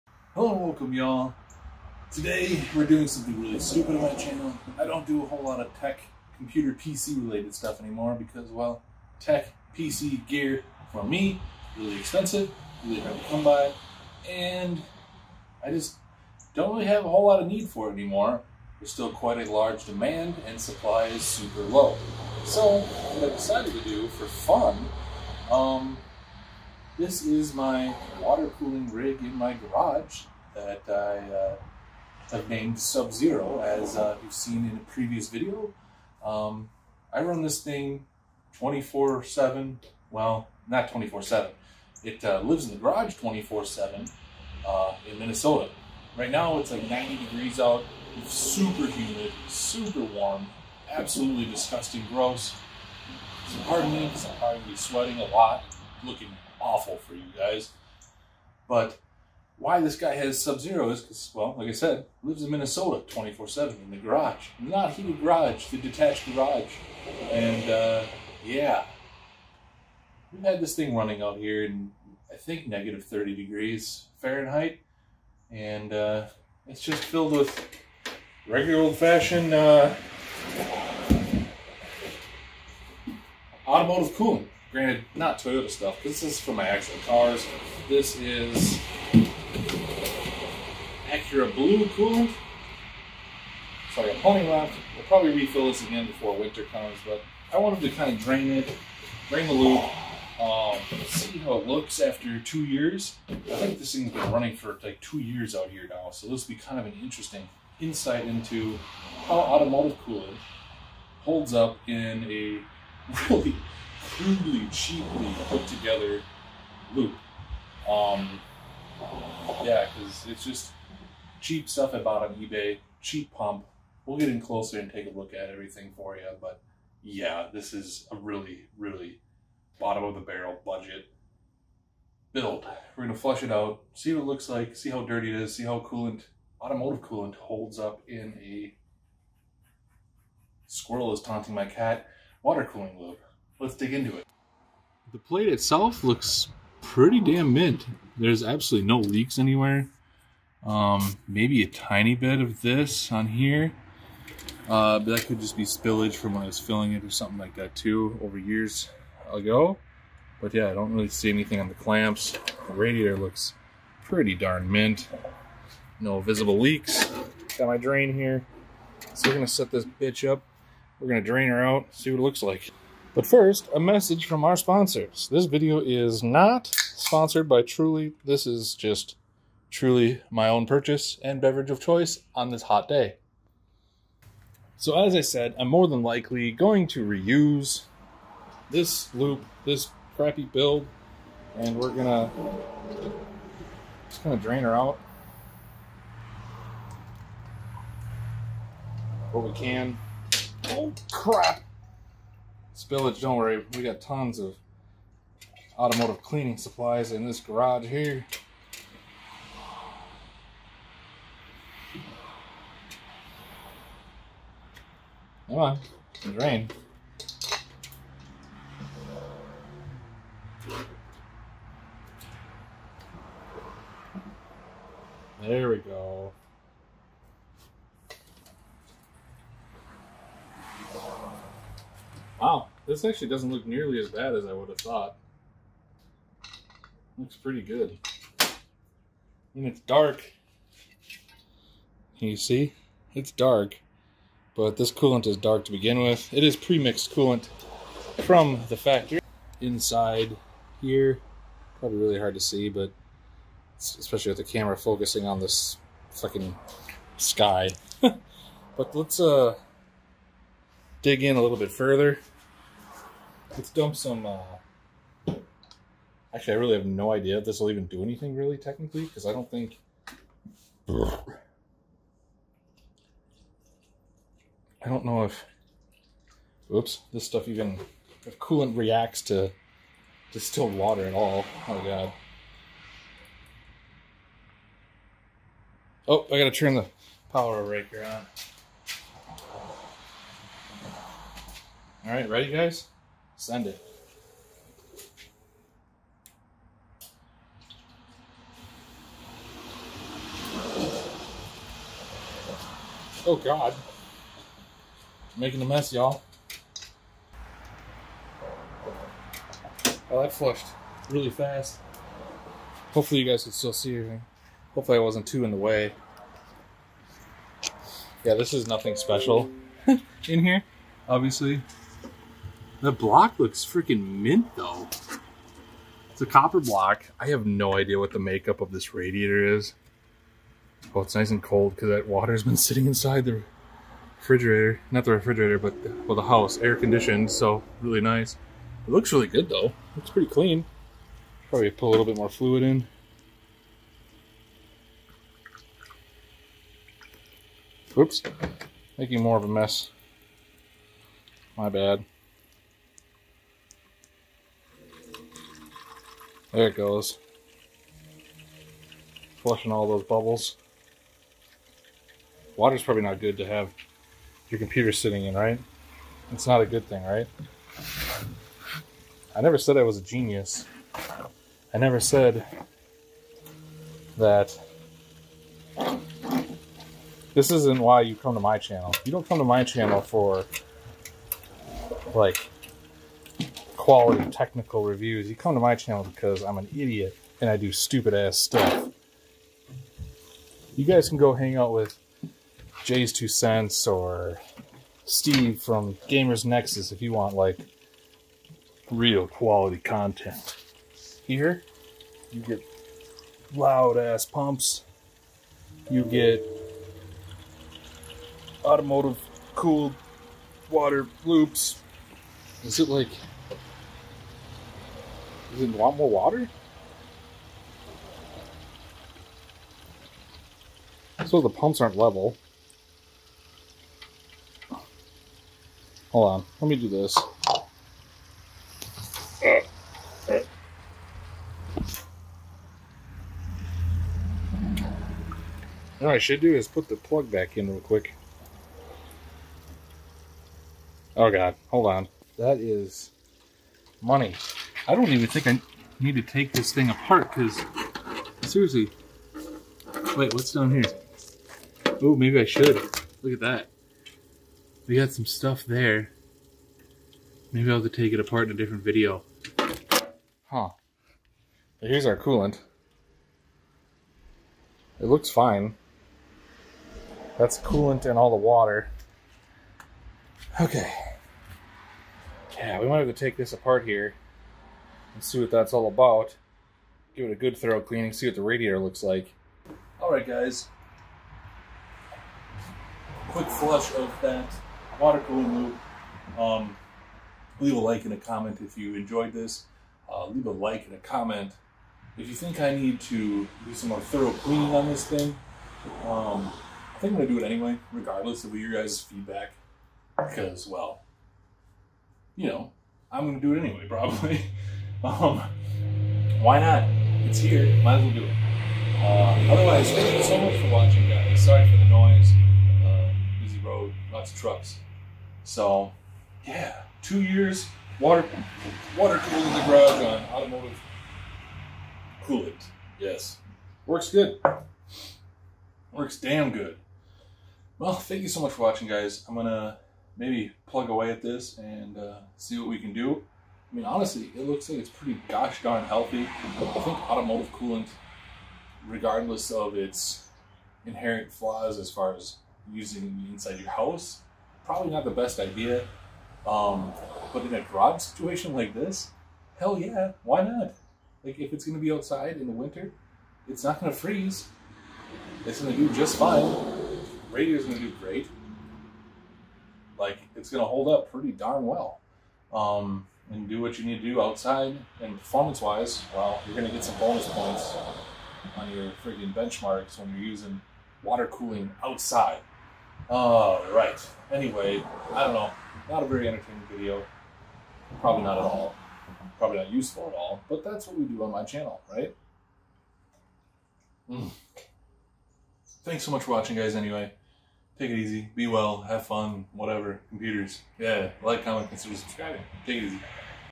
2 Years of Automotive Coolant in a PC How did it hold up?
I have a PC in my garage that I have been running Acura coolant in for 2 years and I wanted to see what the loop looks like now.
3:17 first spill
4:35 second spill
5:45 third spill
6:15 Im not jayztwocents
6:58 fourth spill
7:29 growth?
7:59 Conclusion
Pc Specs
AMD 5800X
GSKILL 32GB RAM
MSI Gaming Trio RTX 3070
MSI X570 Pro Carbon Wifi